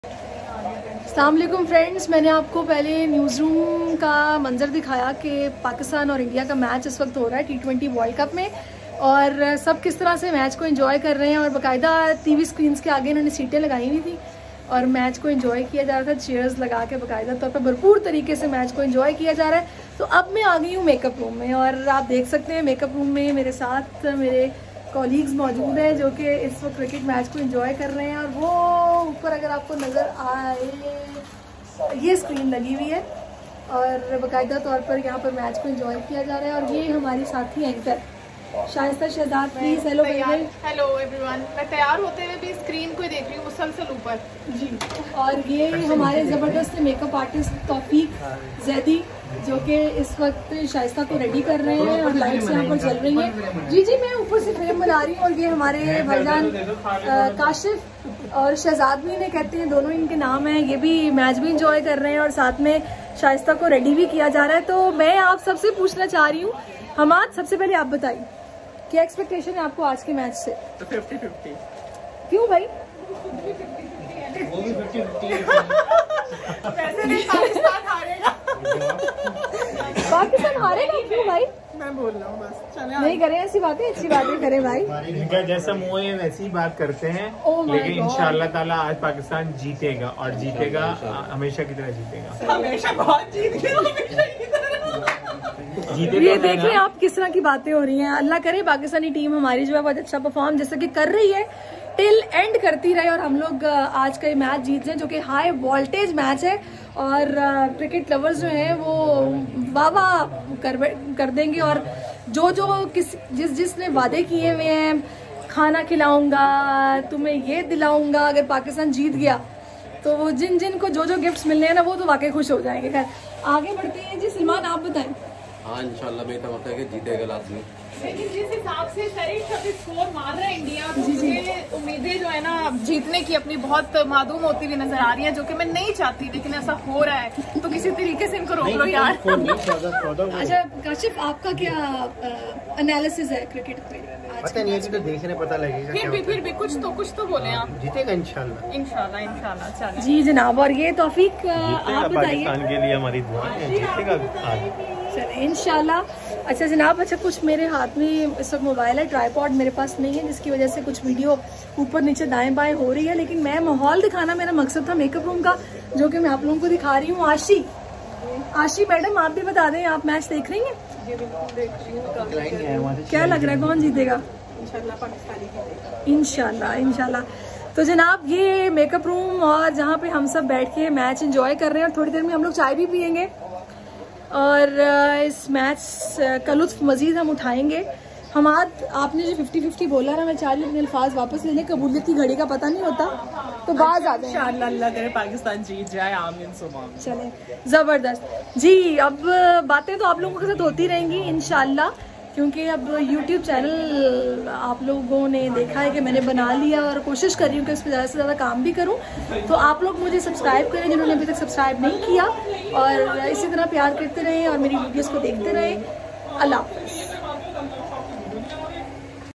Pak india match cricket fever
Enjoying match with my colleagues T20 world cup high voltage match